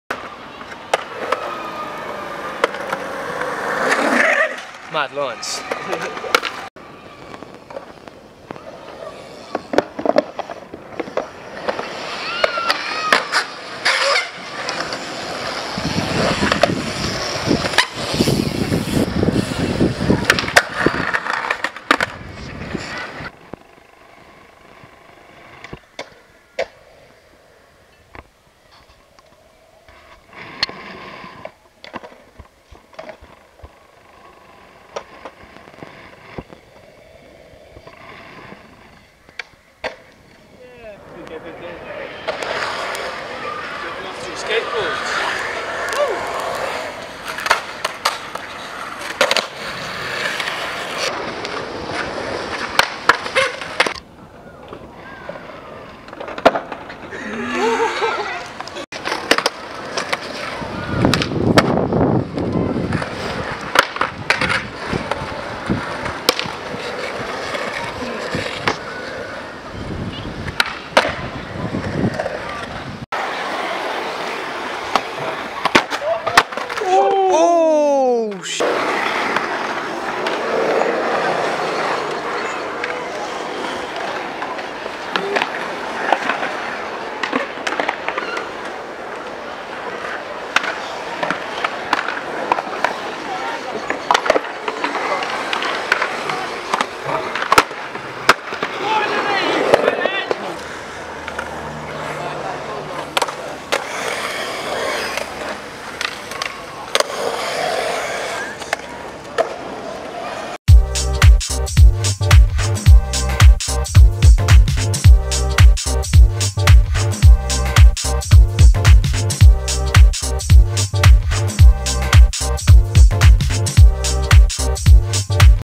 Winchester fun run
Yet another jokes session at Winch with the boys! This place is practically our home now, all the Winch locals are the soundest group i have ever met skateboarding.